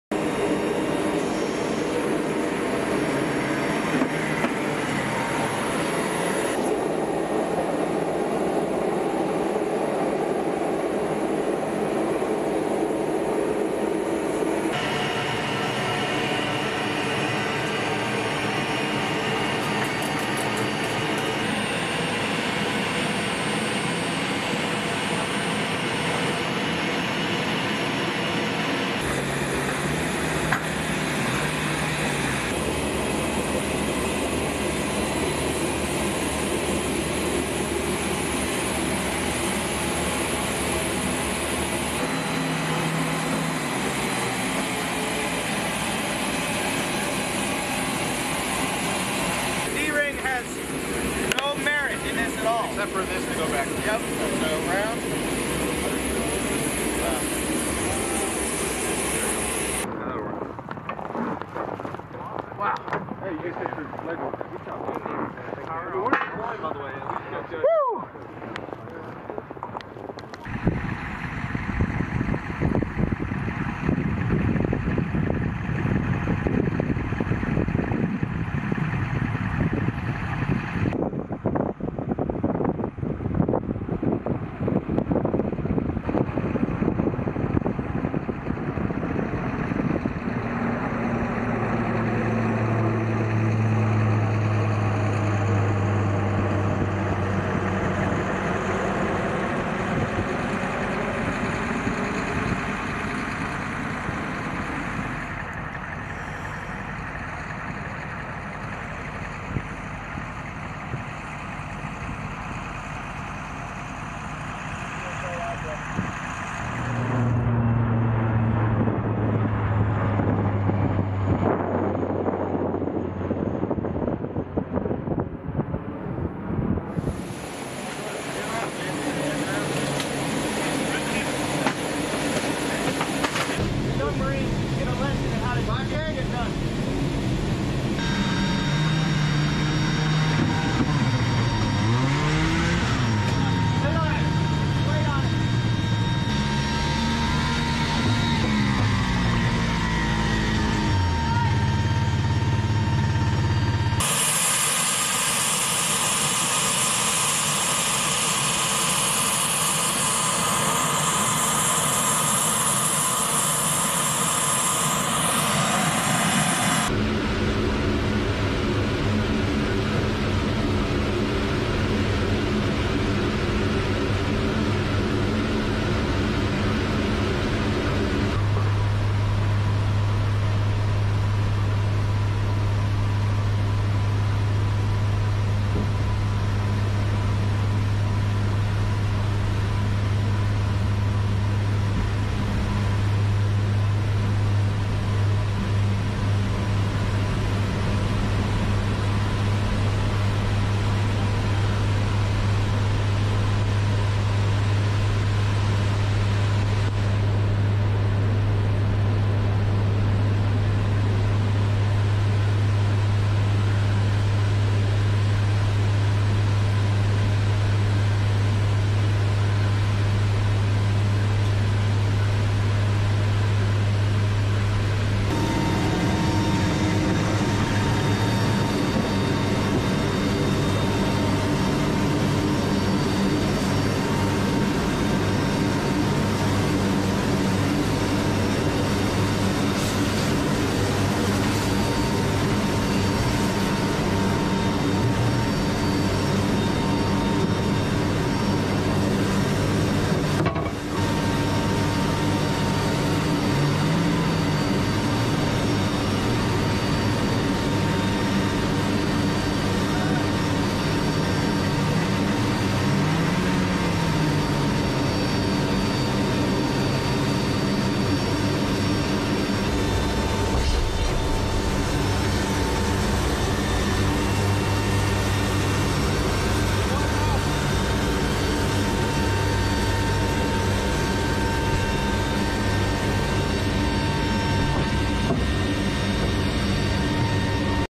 VMGR-152 The Last Frontier (B-Roll)
AK, UNITED STATES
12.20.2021

U.S. Marines with Marine Aerial Refueler Transport Squadron 152, based out of Marine Corps Air Station Iwakuni, Japan, conducted proficiency training in arctic climates while supporting joint operations with the U.S. Army, U.S. Air Force, and the Marine Corps Reserve Toys for Tots program in Alaska. (U.S. Marine Corps Video by Cpl. Jackson Ricker)

Film Credits: Video by Cpl. Jackson Ricker
1st Marine Aircraft Wing